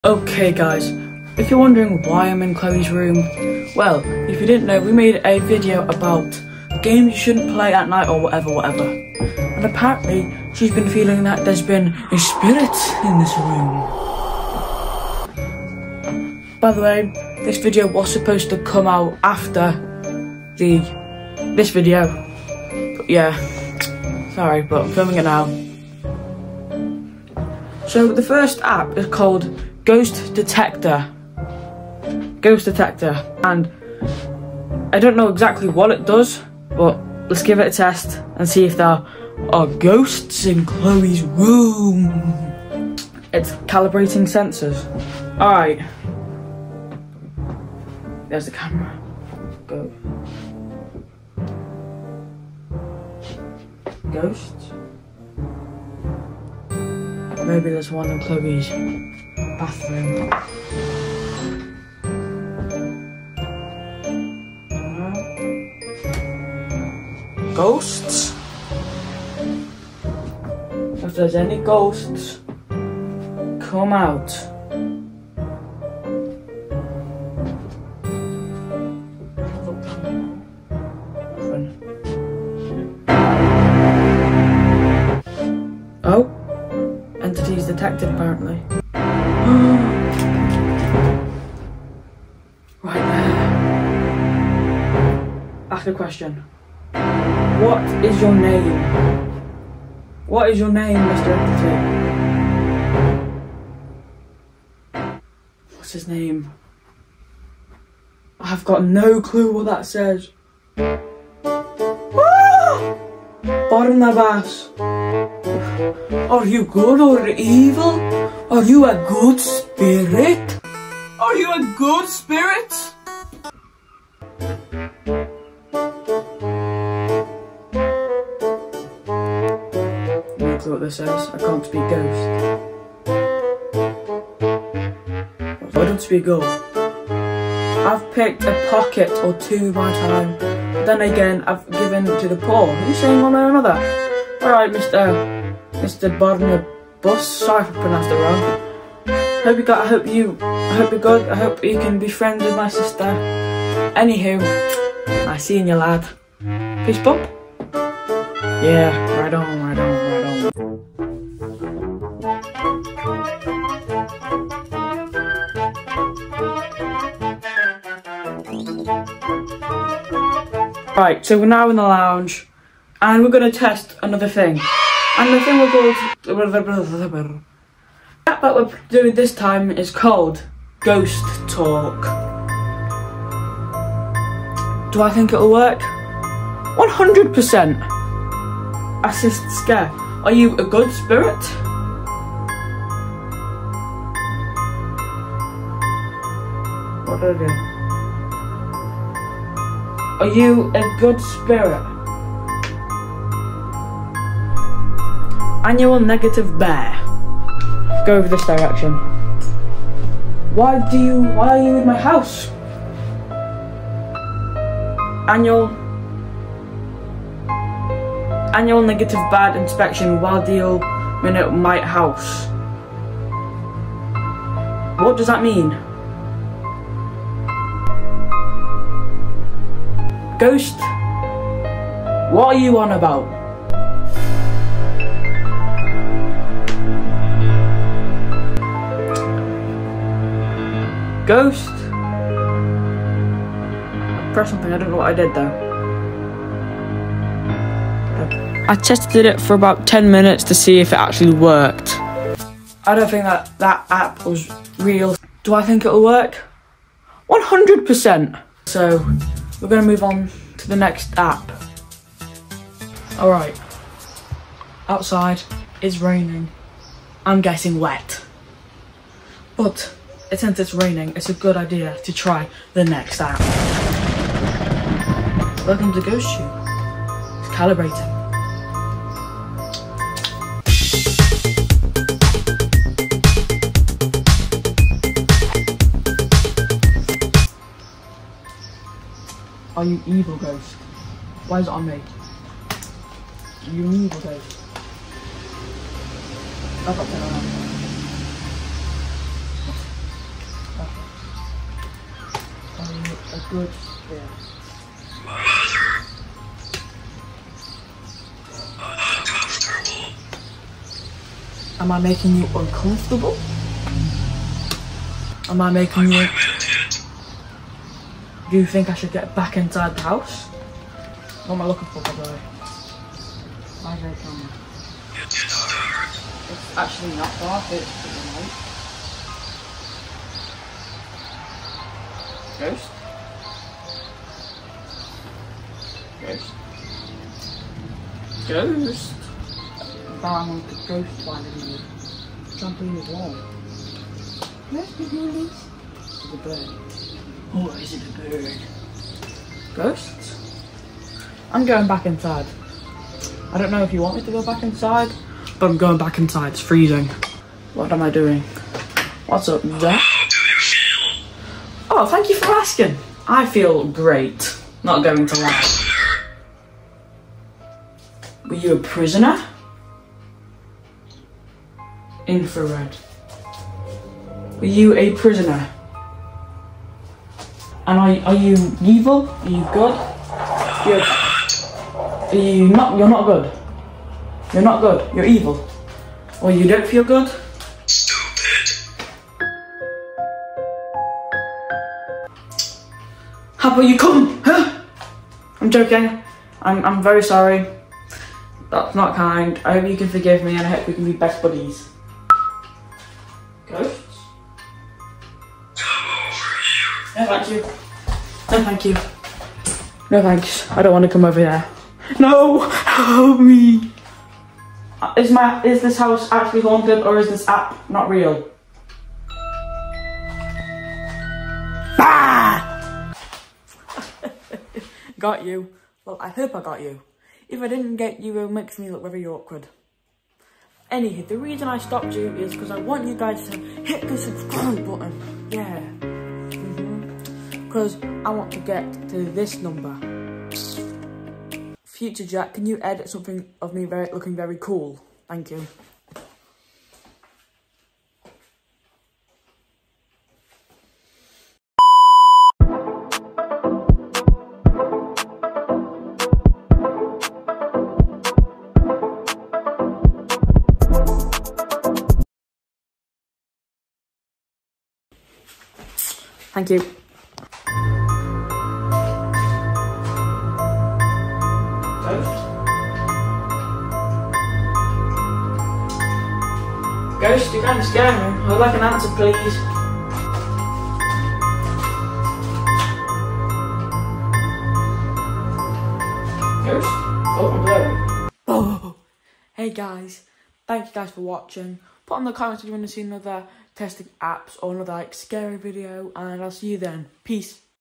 Testing Ghost Hunting Apps!!!
Testing ghost hunting apps to see if they’re real